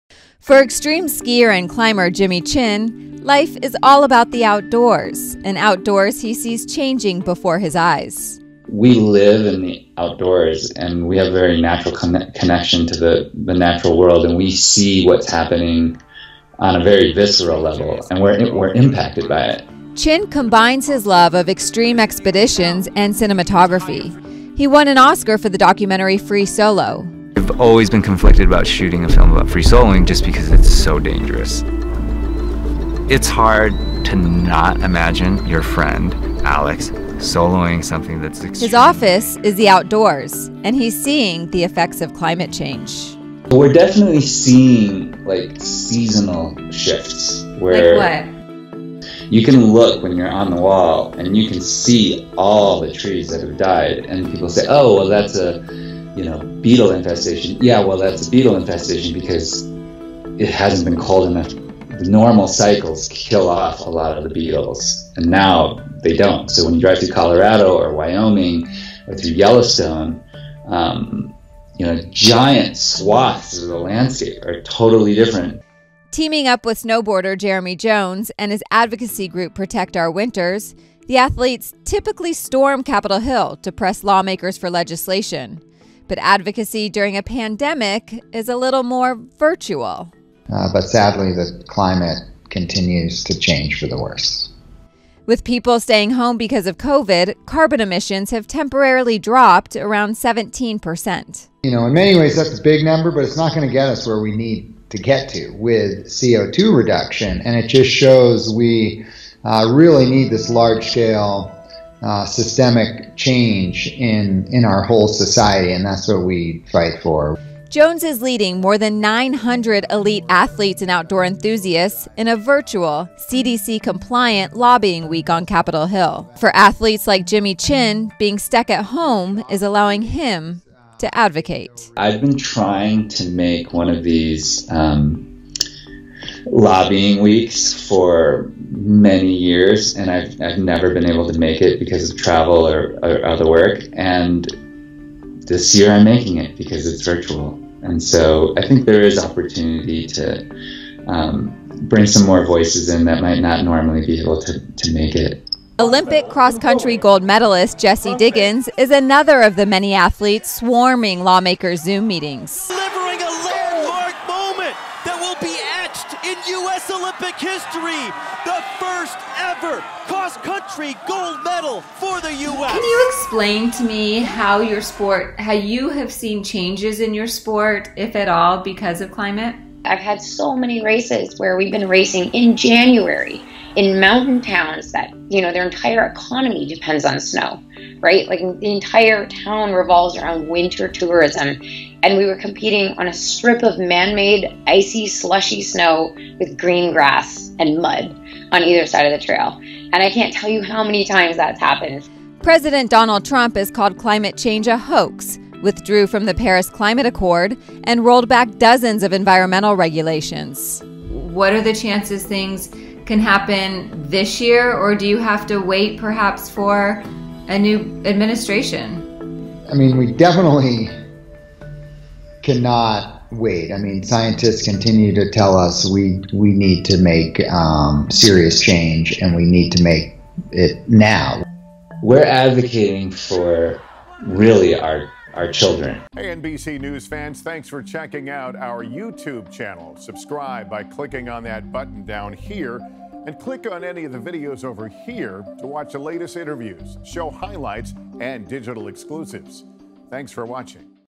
Olympic Athletes Address Climate Change While Quarantined At Home | NBC News NOW
NBC News’ Leigh Ann Caldwell speaks to a group of Olympic athletes who are addressing climate change while quarantined at home.